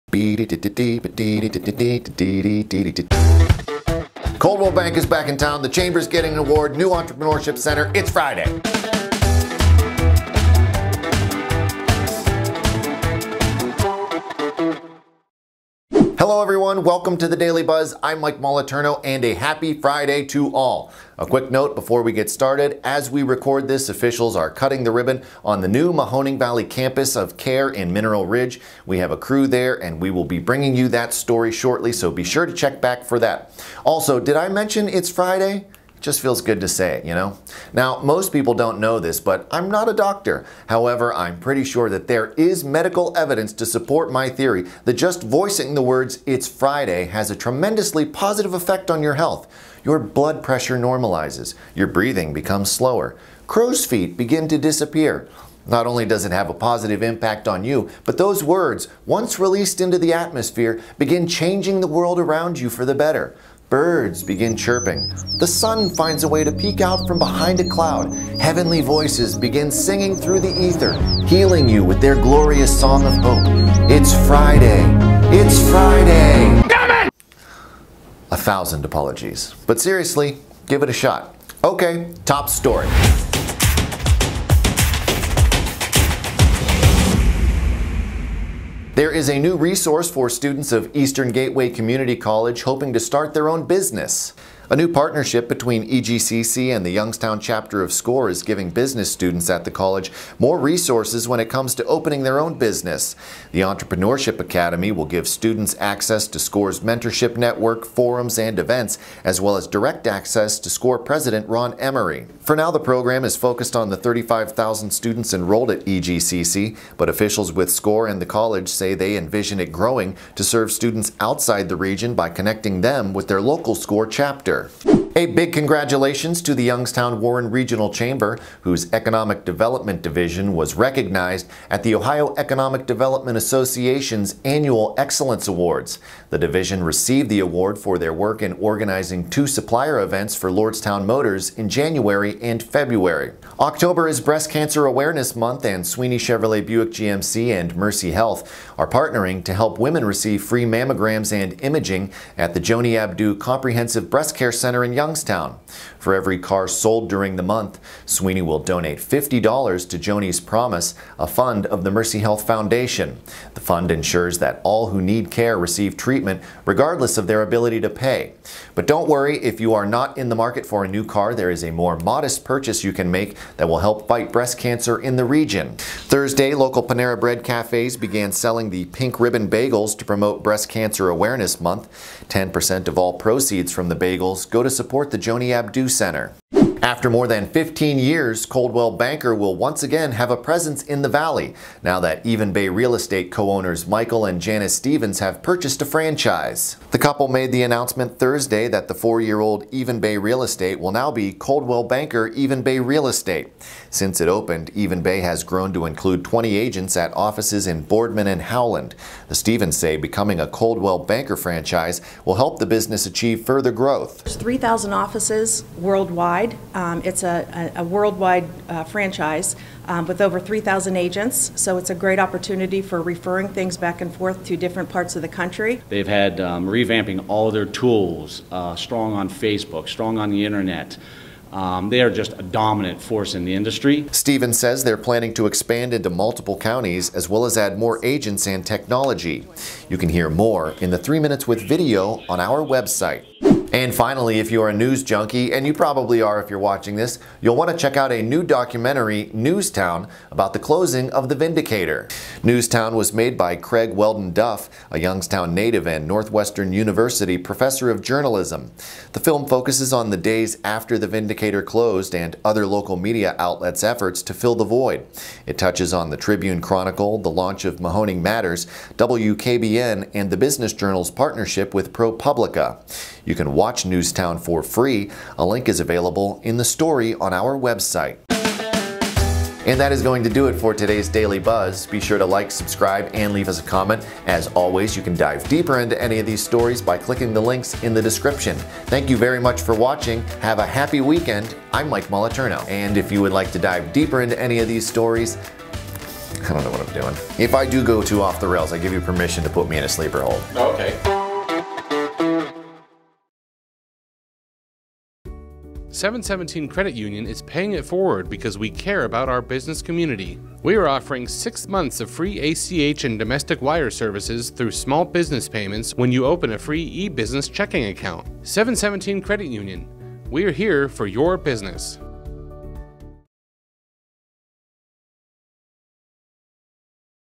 EGCC & Score Partner for Entrepreneurship Academy | Daily Buzz 10-02-20
A new partnership between Eastern Gateway Community College and the Youngstown chapter of Score will provide additional resources to students looking to open their own business.

Plus: How Sweeney Chevrolet Buick GMC, Mercy Health and area Panera Bread stores are joining the fight against breast cancer and IT'S FRIDAY!

With Some Tweaks, Panera Launches Pink Ribbon Bagel Campaign

Coldwell Banker Keeps It in the Family for Return to Valley